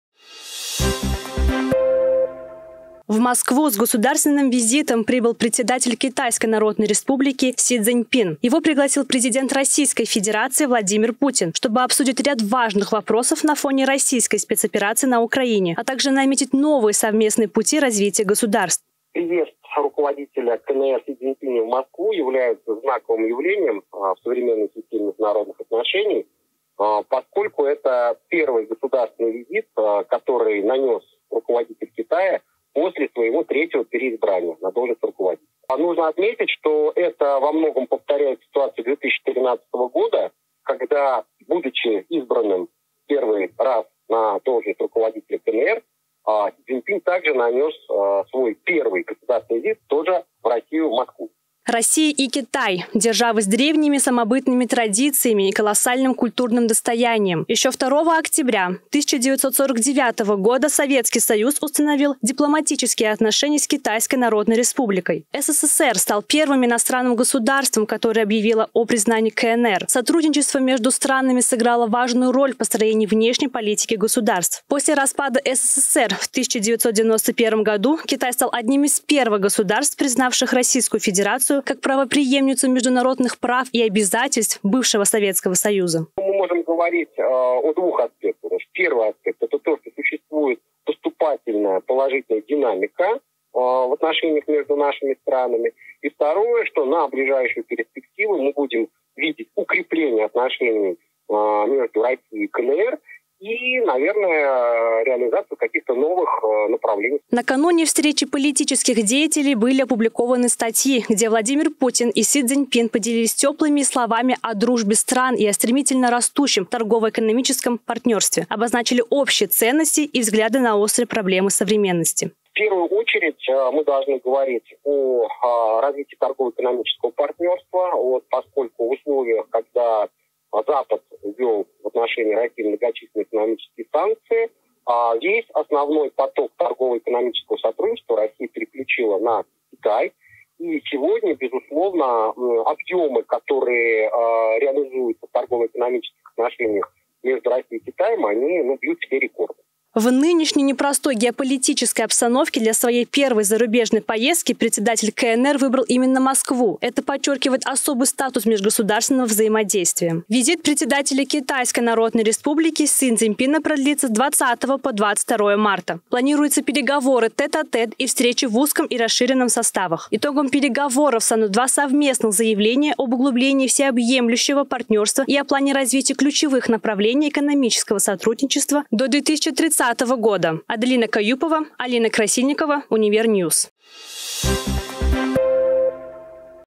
РОССИЯ КИТАЙ: ВИЗИТ СИ ЦЗИНЬПИНА В МОСКВУ
В Москву с государственным визитом прибыл председатель Китайской народной республики Си Цзиньпин. О том, что за собой несёт встреча  – узнаете далее.